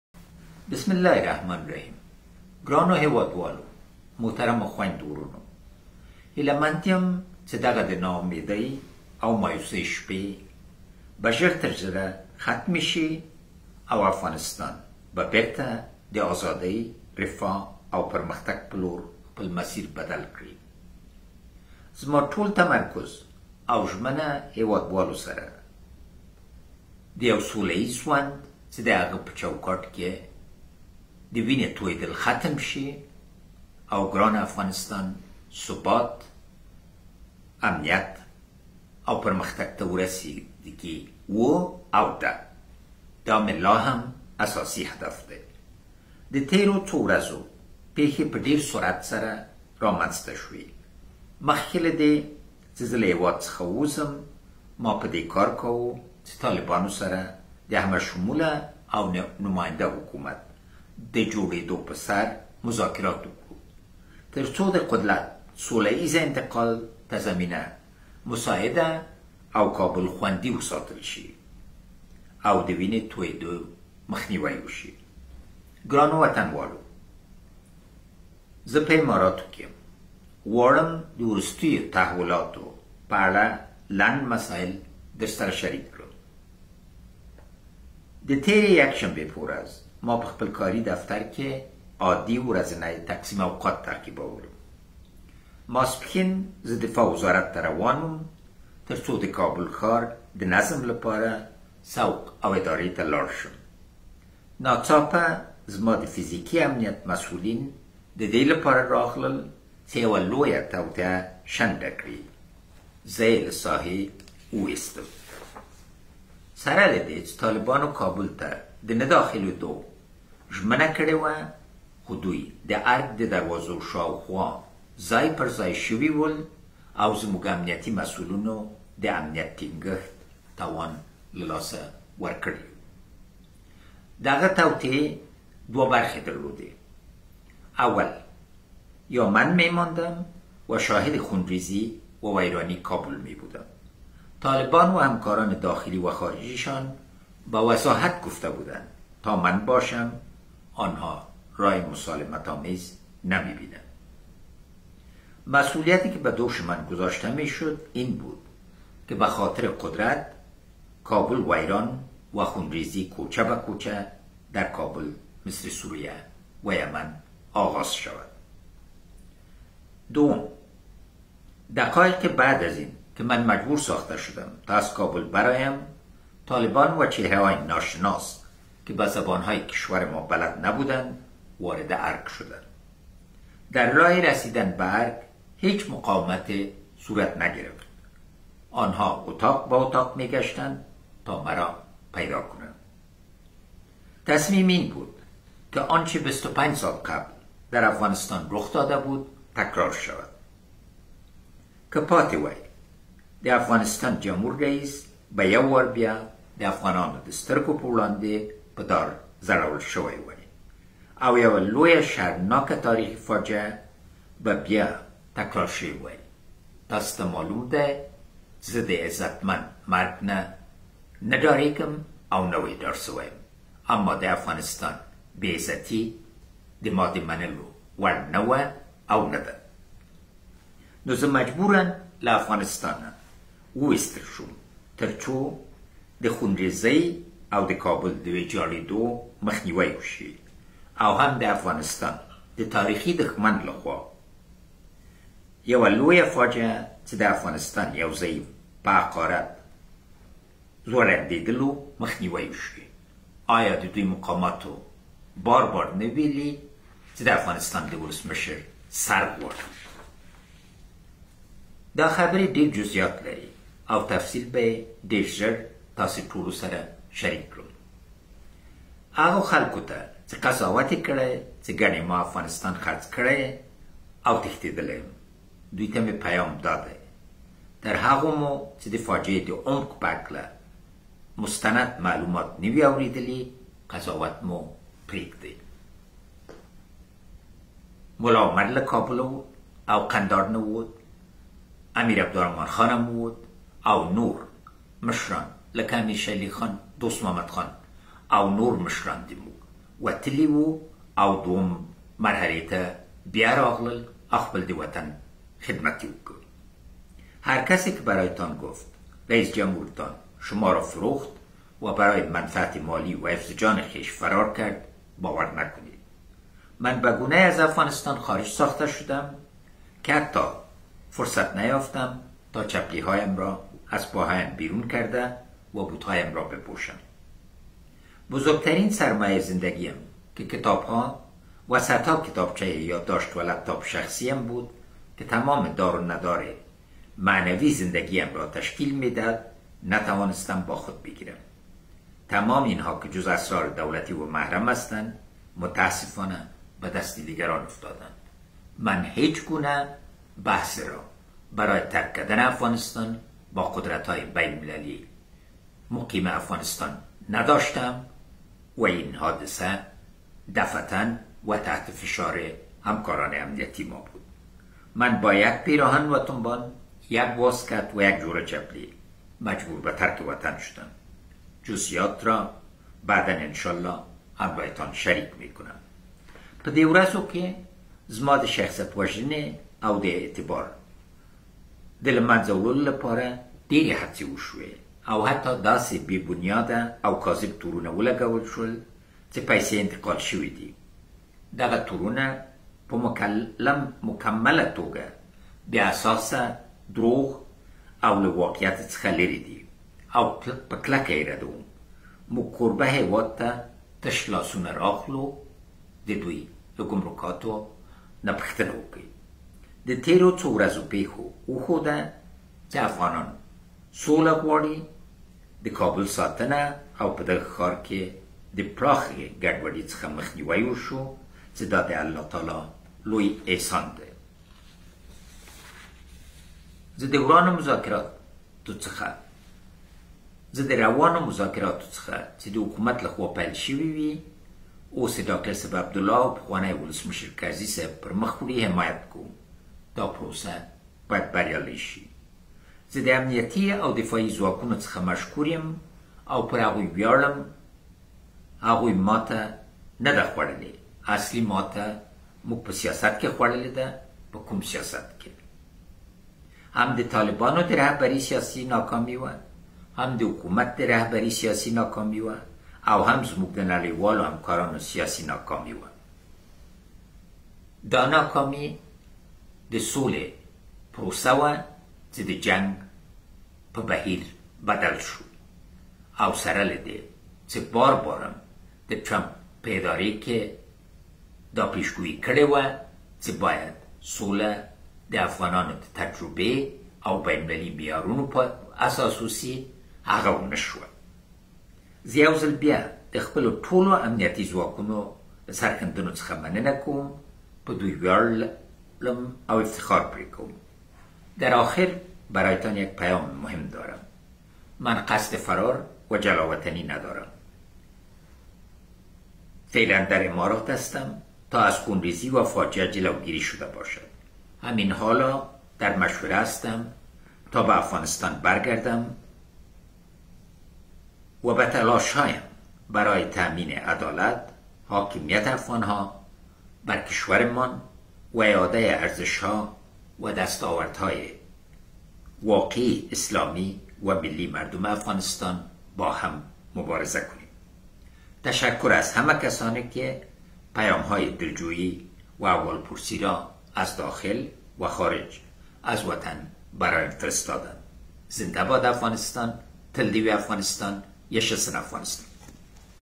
Fugitive President of Afghanistan Ashraf Ghani addresses the nation.
This is Ashraf Ghani's first speech after fleeing Afghanistan in which he is talking to his people about the situation after the Taliban's victory over Kabul.
OnlineWork